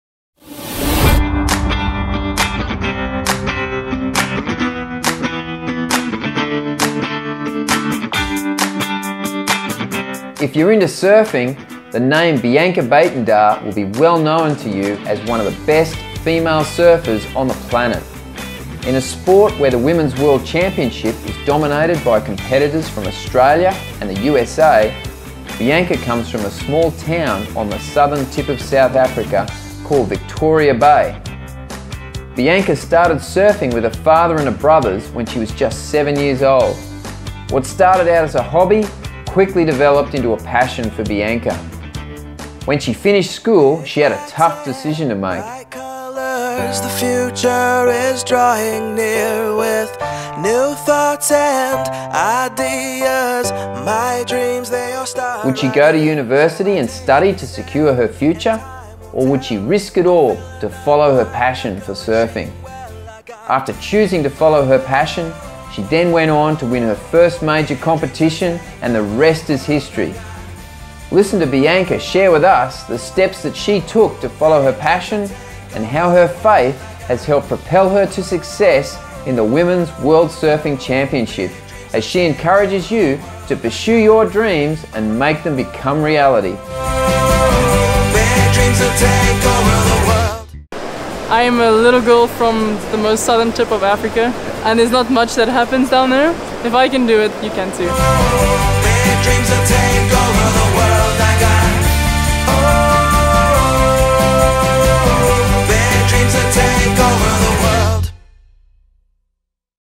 Wild Truth: Surfing with Bianca Buitendag
If you are into Surfing, the name Bianca Baitendar will be well known to you as one of the best female surfers on the planet!
Listen to Bianca share with us, the steps she took to follow her passion and how her faith has helped propel her to success in the Women's World Surfing Championship.